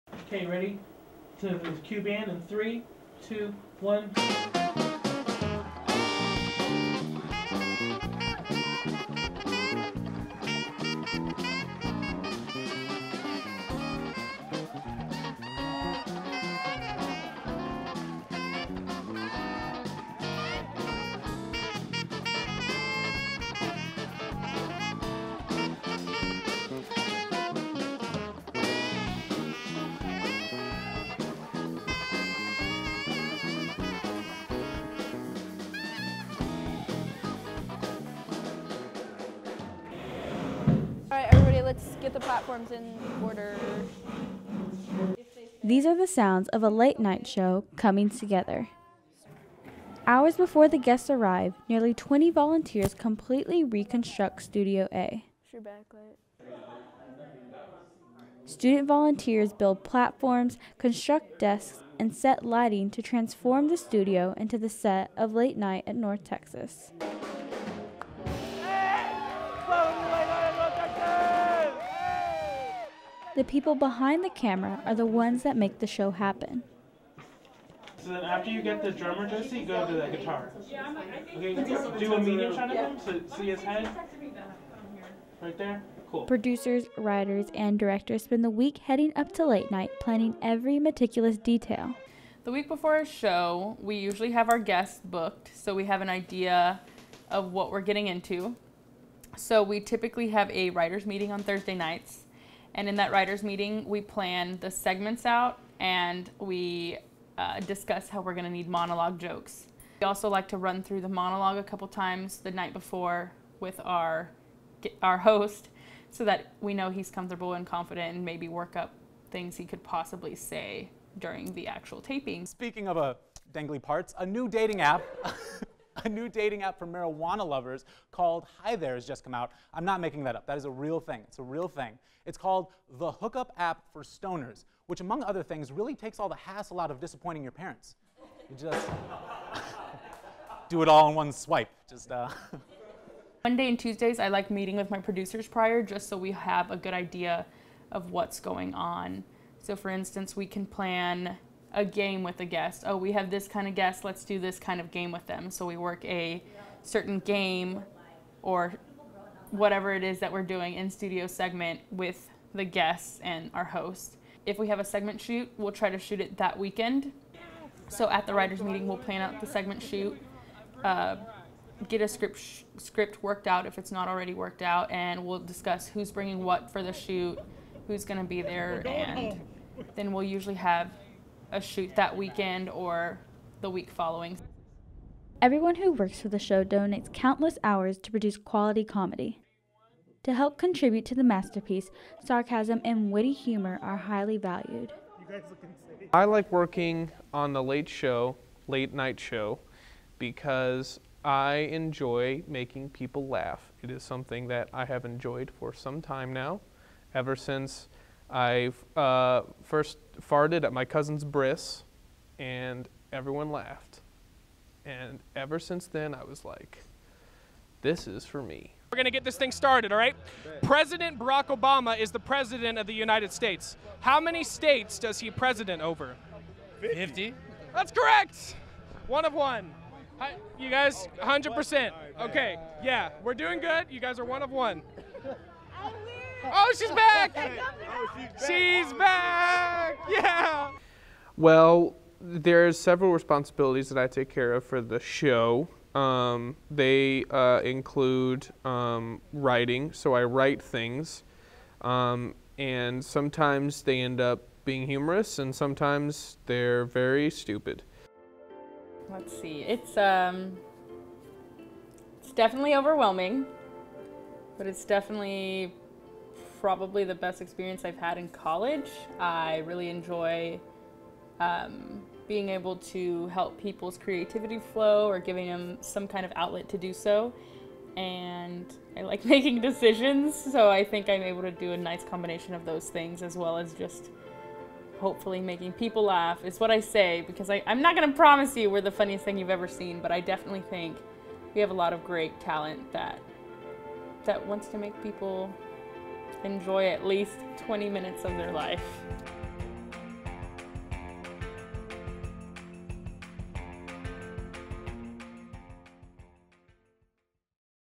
Behind the Scenes of Late Night at North Texas
Behind the scenes of UNT's on campus late night show with the executive producer and head writer.